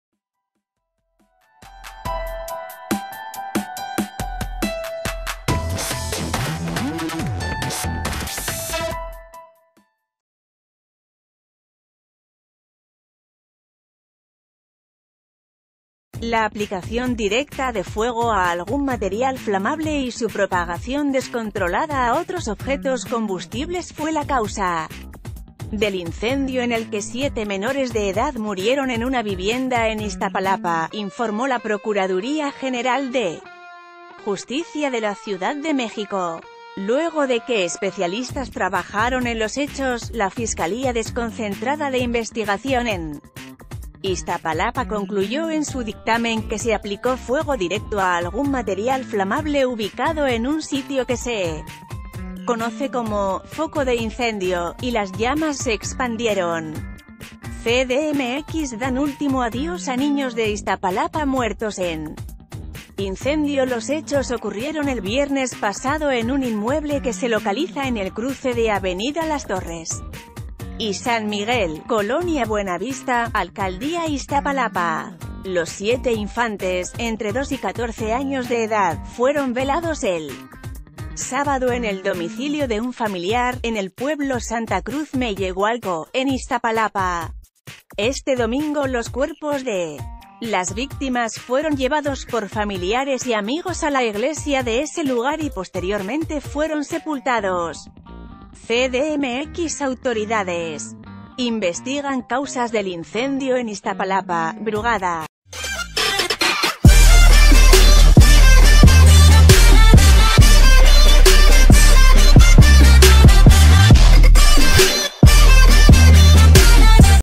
Fuego directo originó incendio en Iztapalapa, concluyen peritos
La aplicación directa de fuego a algún material flamable y su propagación descontrolada a otros objetos combustibles fue la causa del incendio en el que siete menores de edad murieron en una vivienda en Iztapalapa, informó la Procuraduría General de Justicia de la Ciudad de México. Luego de que espe...